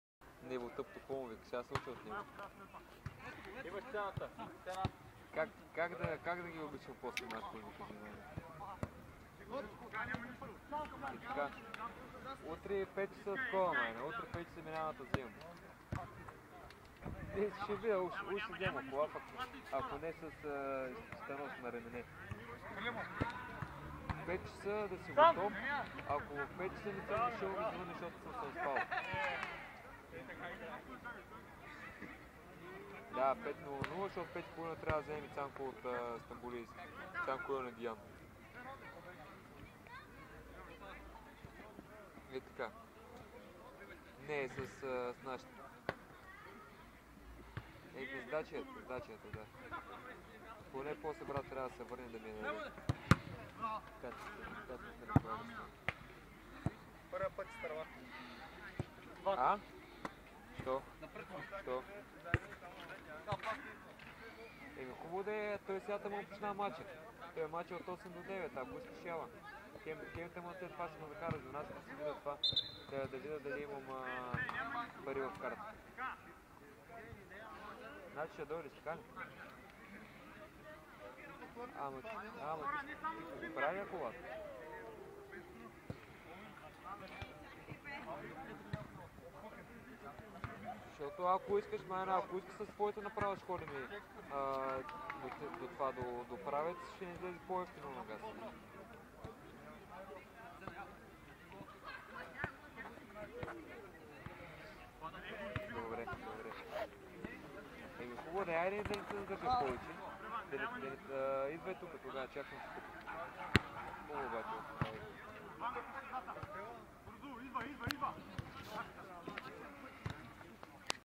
ЖМТ Пчелинчо - ФК Даяна 2:4
Мач от "Висша Лига" - Градска Футболна Лига - Пловдив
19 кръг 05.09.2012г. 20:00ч
Спортен център "Арена" (Пловдив)
Видео от мача